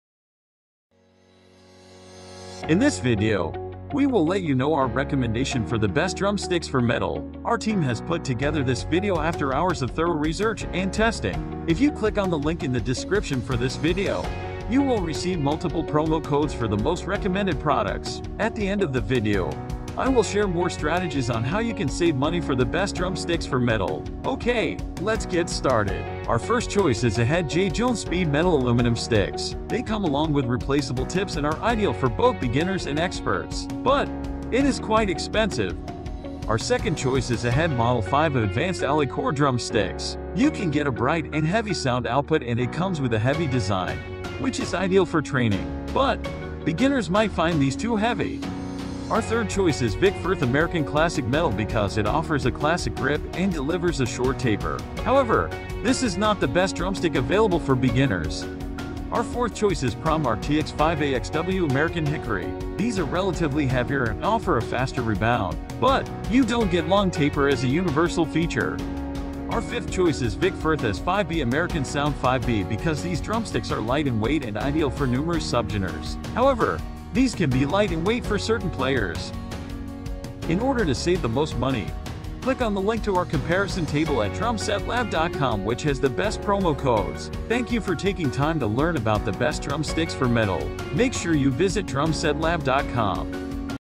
BEST DRUMSTICKS FOR METAL
We’re going to discuss the pros and cons of All The BEST DRUMSTICKS FOR METAL

Here are the details:
1.  Ahead JJ1 SPEED METAL Aluminum Sticks
2. Ahead Model 5A Advanced Alloy Core Drum Sticks
3. Vic Firth American Classic Metal
4. Promark TX5AXW American Hickory
5. Vic Firth AS5B American Sound 5B